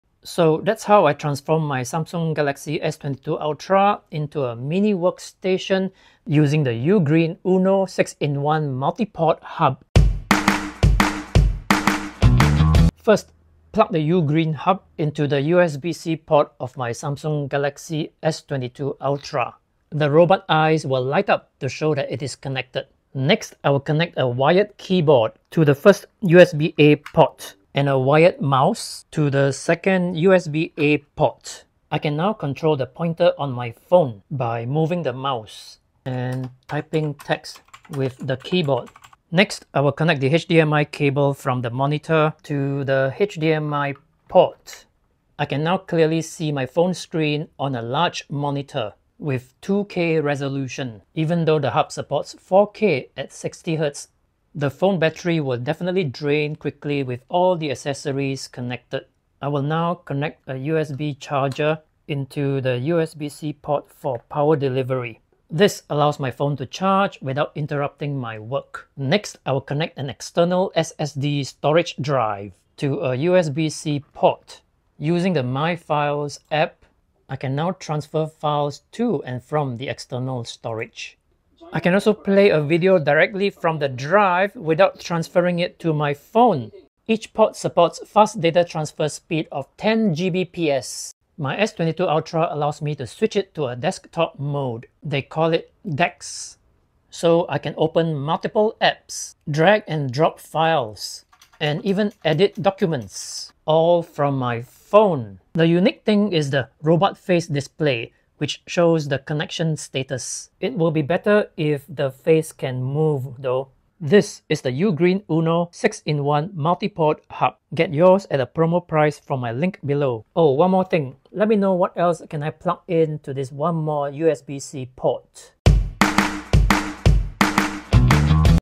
How to Set Up a Mini Workstation with a Samsung S22 Ultra and the Ugreen Uno 6-in-1 USB-C Hub
In this video, I will show you how to transform your Samsung Galaxy S22 Ultra into a mini workstation using the Ugreen Uno 6-in-1 multiport hub.

This hub is a great way to add extra ports and functionality to your phone, making it more versatile and powerful.

Here are the steps I will follow:

1. Plug the Ugreen Hub into the USB-C port of your Samsung Galaxy S22 Ultra.
2. Connect a wired keyboard to the first USB-A port and a wired mouse to the second USB-A port.
3. Connect the HDMI cable from the monitor to the HDMI port.
4. Connect a USB charger to the USB-C port for power delivery.
5. Connect an external SSD storage drive to a USB-C port.

Once you have completed these steps, you will be able to use your Samsung Galaxy S22 Ultra as a mini workstation. You can open multiple apps, drag and drop files, and even edit documents all from your phone.

The Ugreen Uno 6-in-1 multiport hub is a great way to expand the capabilities of your Samsung Galaxy S22 Ultra. If you are looking for a way to make your phone more versatile, I highly recommend this product.

Here are some of the benefits of using the Ugreen Uno 6-in-1 multiport hub:

* It is compact and portable, so you can take it with you wherever you go.
* It is compatible with a variety of devices, including the Samsung Galaxy S24 Ultra, Apple MacBook, and Windows Laptop.
* It has a variety of ports, including 2x USB-C, 2x USB-A, 1x HDMI, and 1x USB-C PD.
* It is affordable and offers great value for money.

If you are interested in learning more about the Ugreen Uno 6-in-1 multiport hub, check it out

~ Adrian Lee